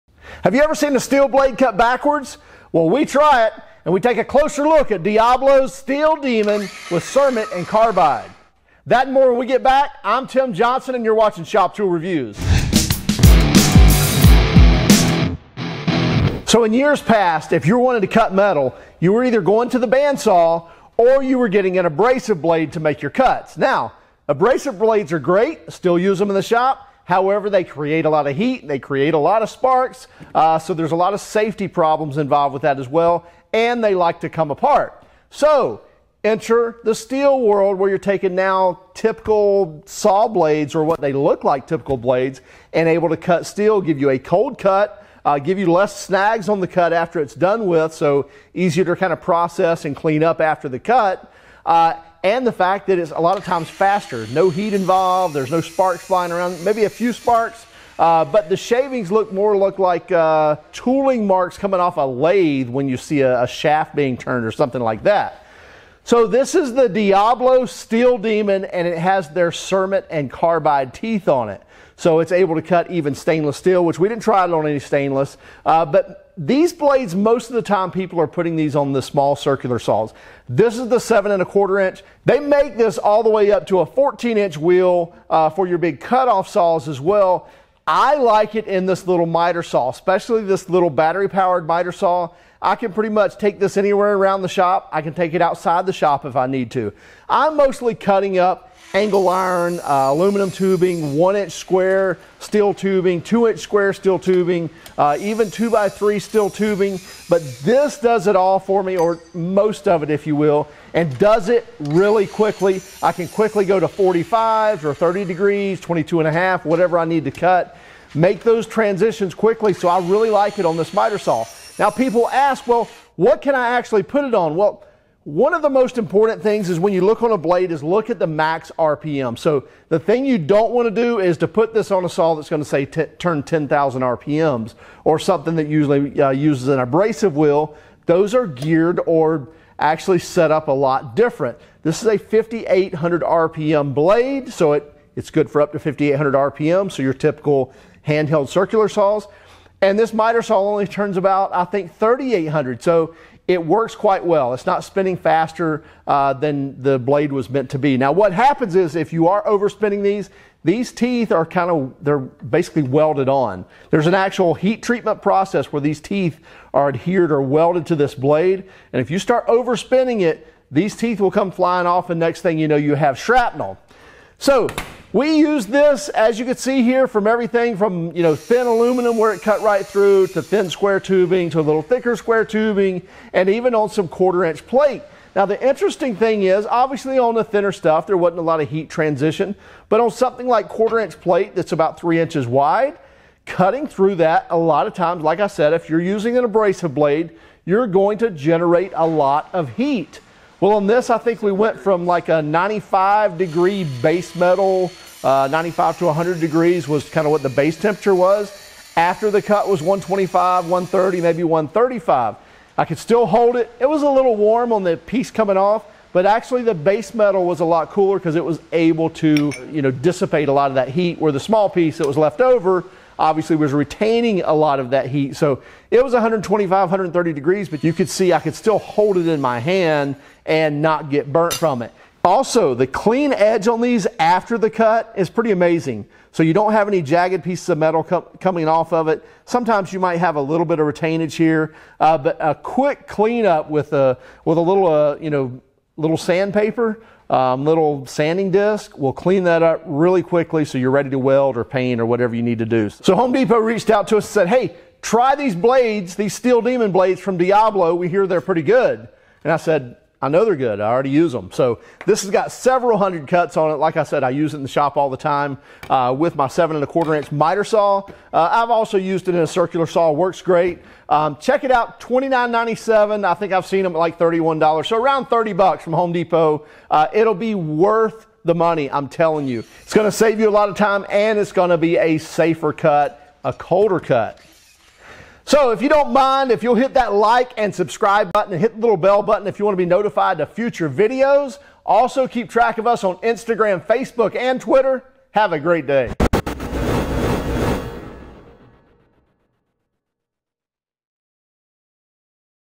Diablo Steel Demon Cermet and Carbide Metal Saw Blade Video
It's so hard to believe that a blade that looks like a wood blade will cut through plate steel with very little effort.